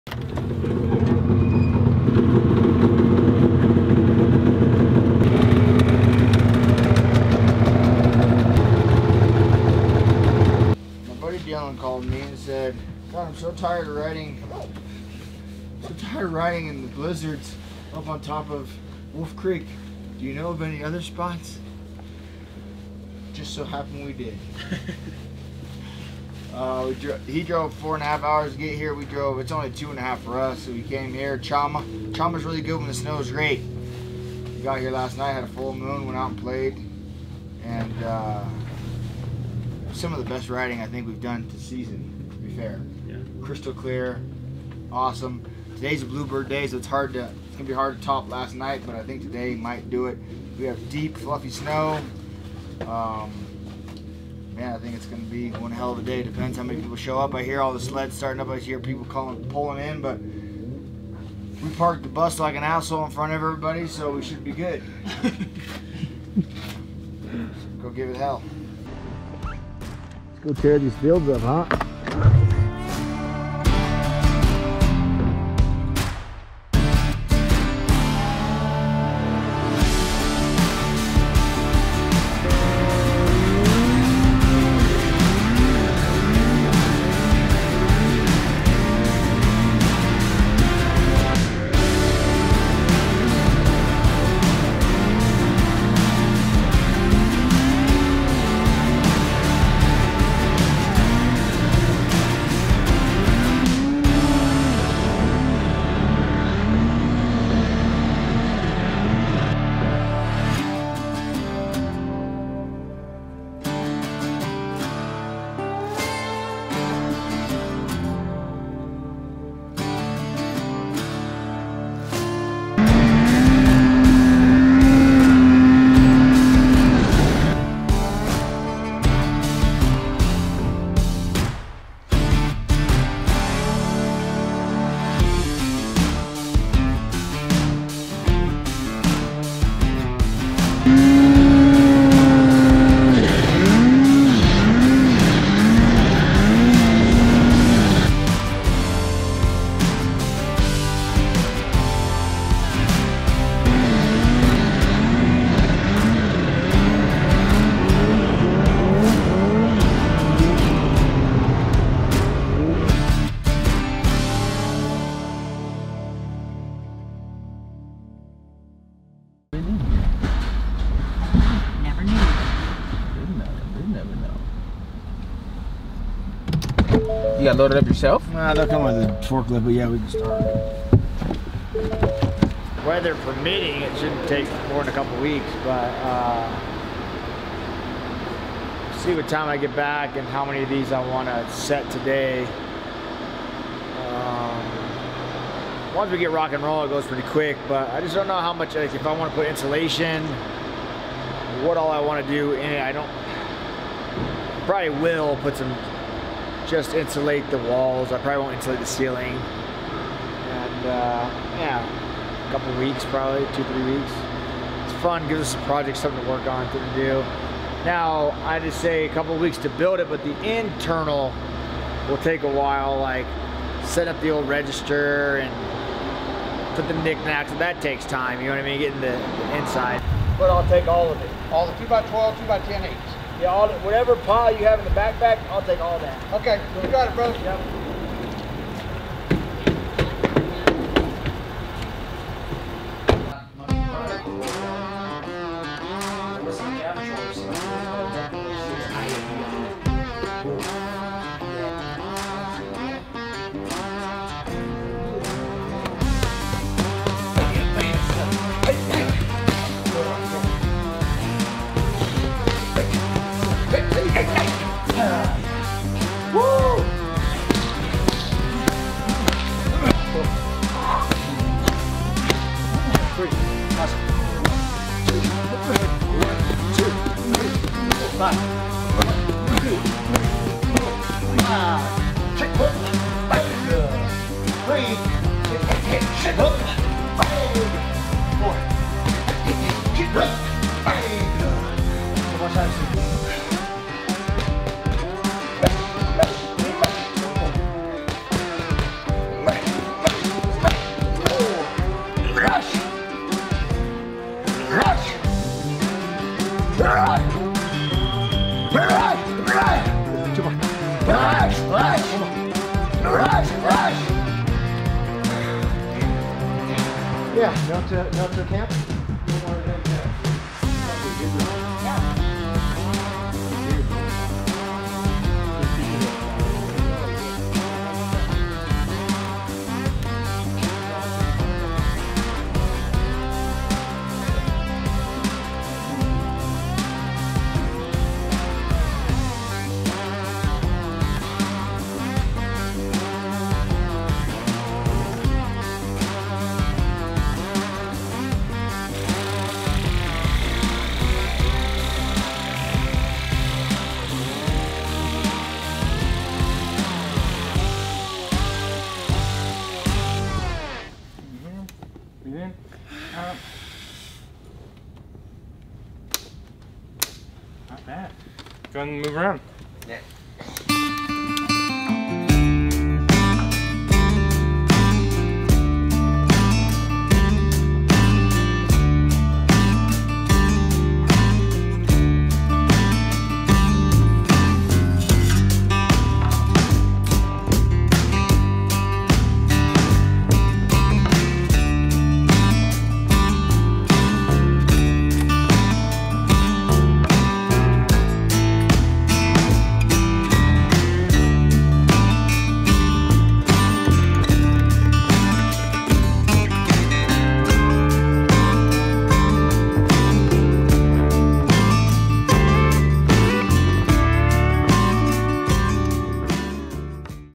Cowboy Goes for a RIP and we keep building the BMFranch.com General Store
Yes! The Diego fight has been announced but before that happened we have been busy doing quite a bit on and off the ranch.

Ripping some sleds thanks to @VPRacingFuel and smashing some pads with (IG) @Coachrayyee is always a hell of a good time and at the end of the day, the first walls of the BMFRanch.com general store are going up